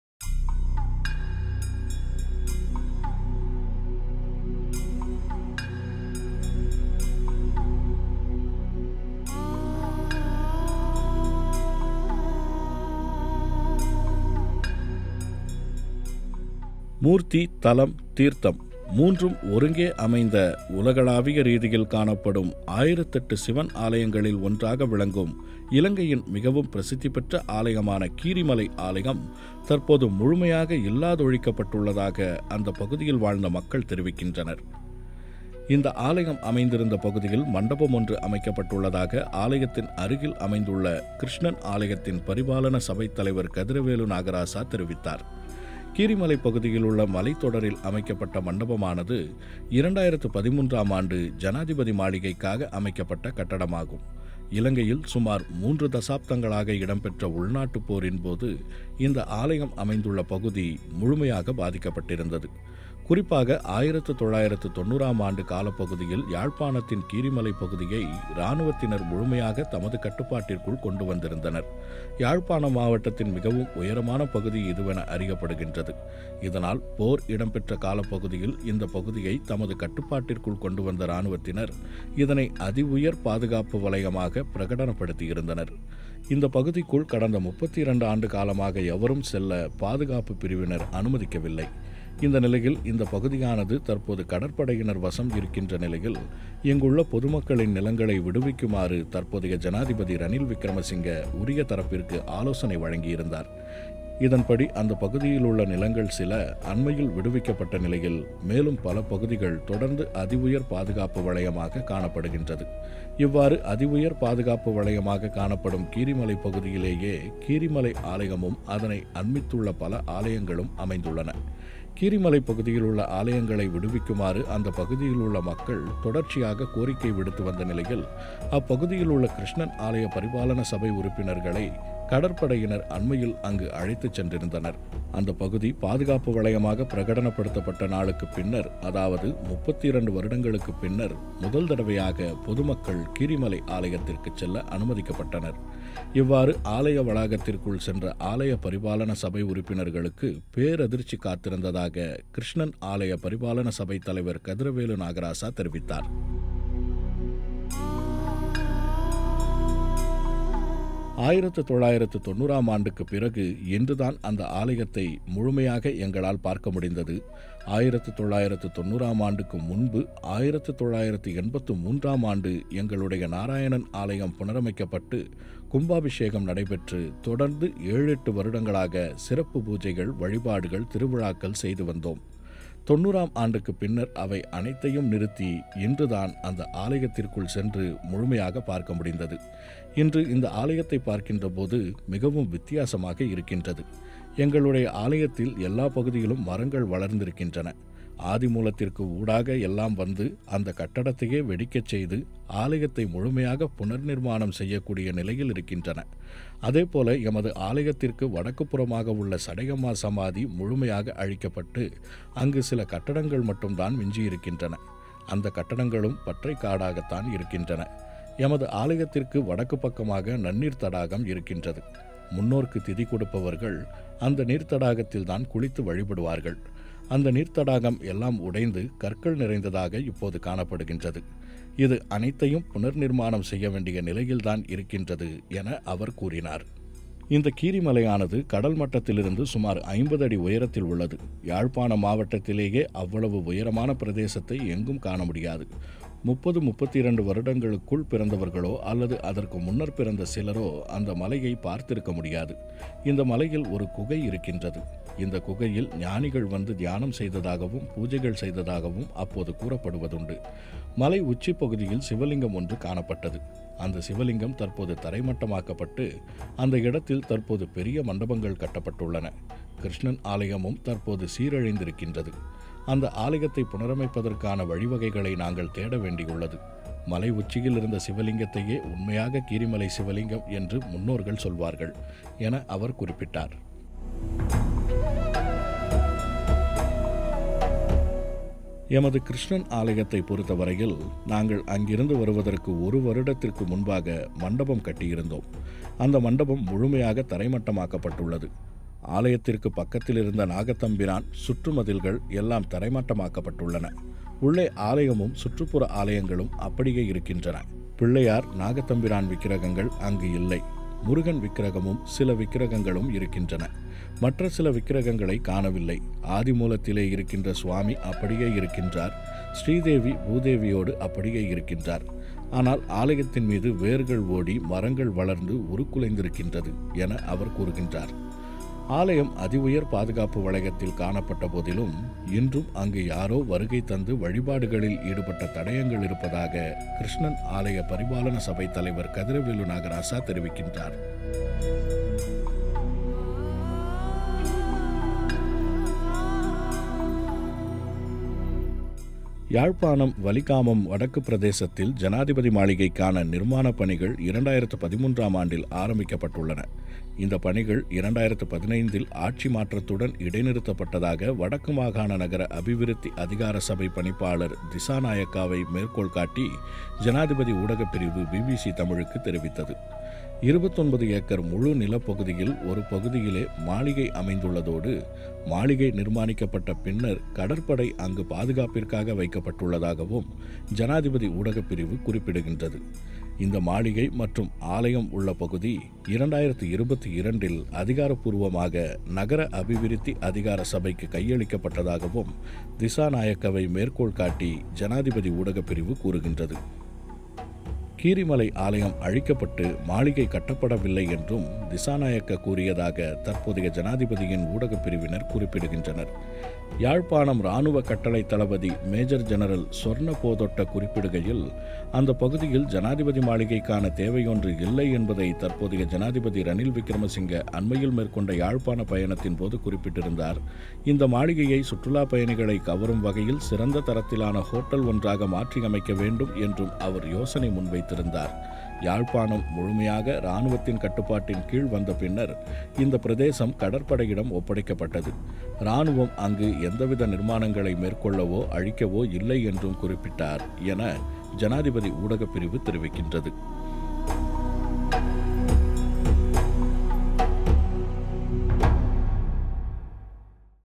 Sri Lanka கீரிமலை சிவன் ஆலயம்: 32 ஆண்டுகள் யாரும் வழிபடாத இந்த கோயில் இப்போது எப்படி இருக்கிறது?
மூர்த்தி, தலம், தீர்த்தம் மூன்றும் ஒருங்கே அமைந்த உலகளாவிய ரீதியில் காணப்படும் ஆயிரத்தெட்டு சிவன் ஆலயங்களில் ஒன்றாக விளங்கும், இலங்கையின் மிகவும் பிரசித்திப் பெற்ற ஆலயமான கீரிமலை ஆலயம் தற்போது முழுமையாக இல்லாதொழிக்கப்பட்டுள்ளதாக அந்த பகுதியில் வாழ்ந்த மக்கள் தெரிவிக்கின்றனர்.